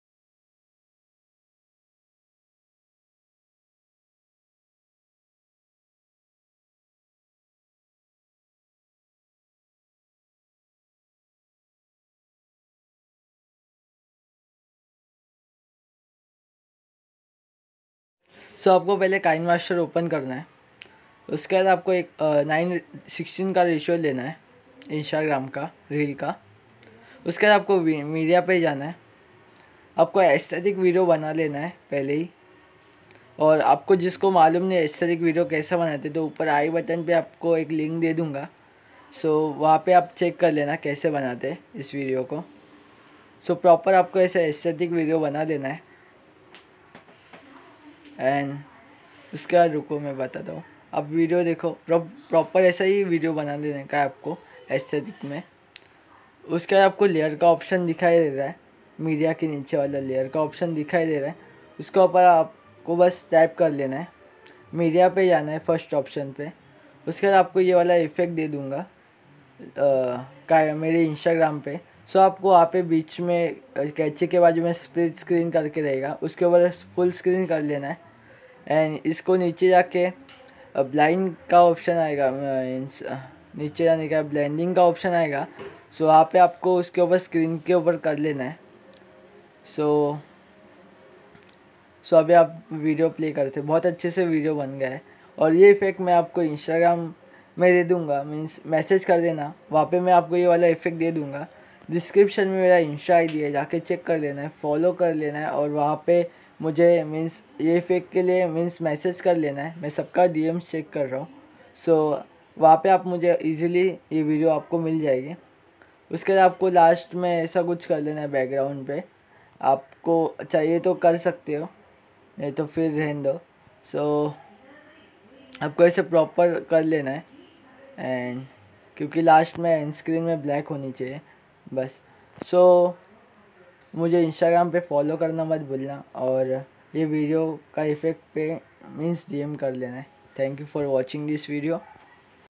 How to add rebooting effect in your Instagram reel / new Instagram aesthetic video tutorial in hindi
How to make creative Instagram reel.
Suggest me more videos from tutorial on Instagram...
DM on Instagram for this effect...